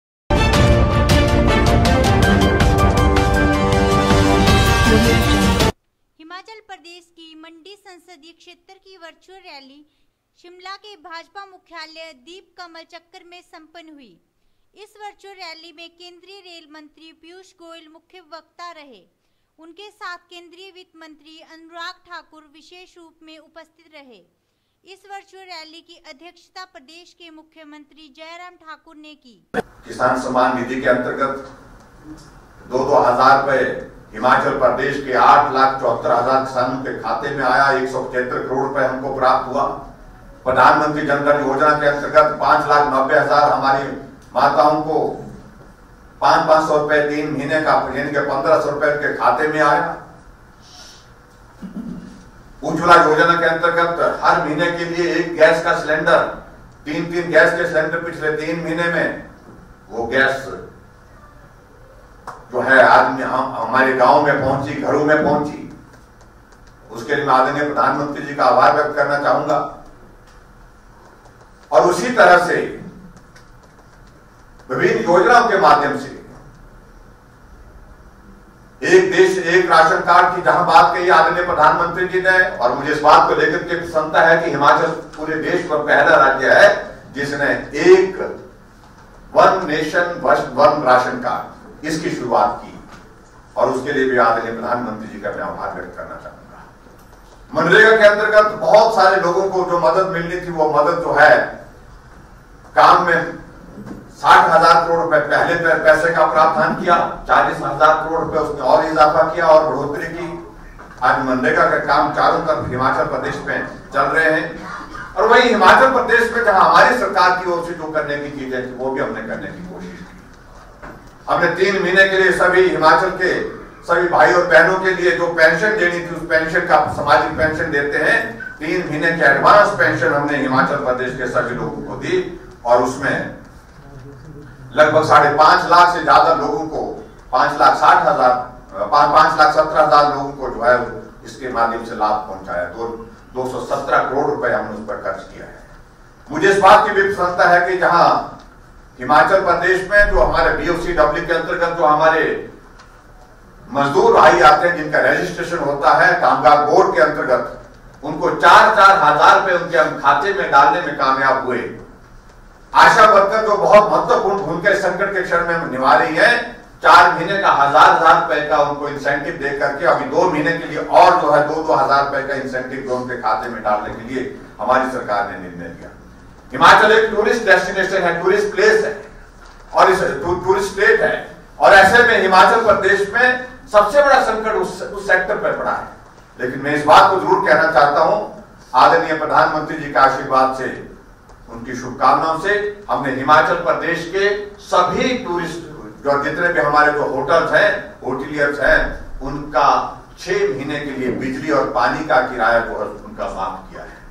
हिमाचल प्रदेश की मंडी संसदीय क्षेत्र की वर्चुअल रैली शिमला के भाजपा मुख्यालय दीप कमल चक्कर में संपन्
हिमाचल प्रदेश की मंडी संसदीय क्षेत्र की वर्चुअल रैली शिमला के भाजपा मुख्यालय दीप कमल चक्कर में संपन्न हुई इस वर्चुअल रैली में केंद्रीय रेल मंत्री पीयूष गोयल मुख्य वक्ता रहे